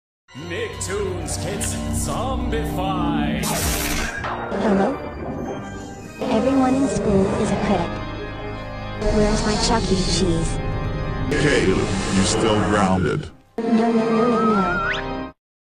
Caillou gets Zombified!! [FANMADE/INSPIRED!]
Caillou has been zombified! HAPPY HALLOWEEN!

This is my own inspired Zombified Bumper I created for Halloween with Wrapper, and Wevideo! I hope you like it!! and also, message to the folks who found the Nicktoons Zombified Bumpers, it's time to Congrats yourselves! Mission Accomplish!! Have a Zombified Halloween Everyone! Muahahahahahahahahahahaha!!!

(BTW: I had to use Ivy Voice because I so fricking pissed off after the voiceforge link got expired!)

There is so much to do, all here in RameshJR2006!